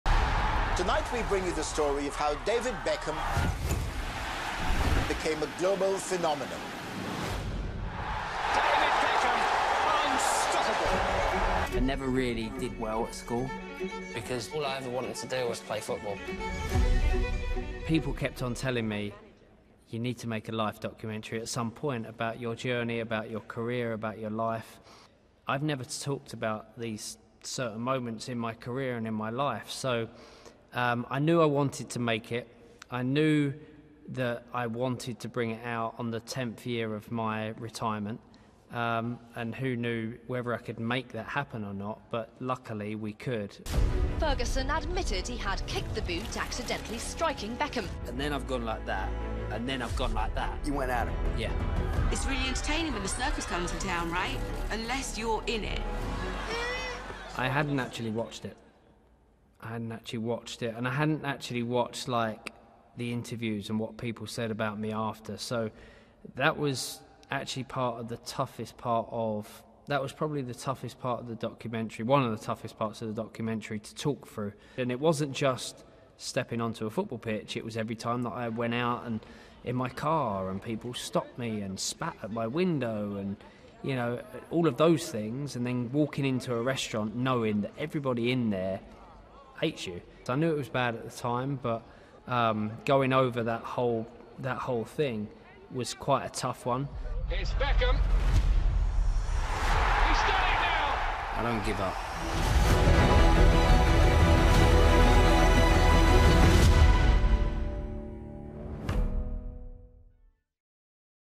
David Beckham reveals 'clinical depression' after World Cup red card in Netflix documentary
David Beckham's new documentary series launches on Netflix on 4 October.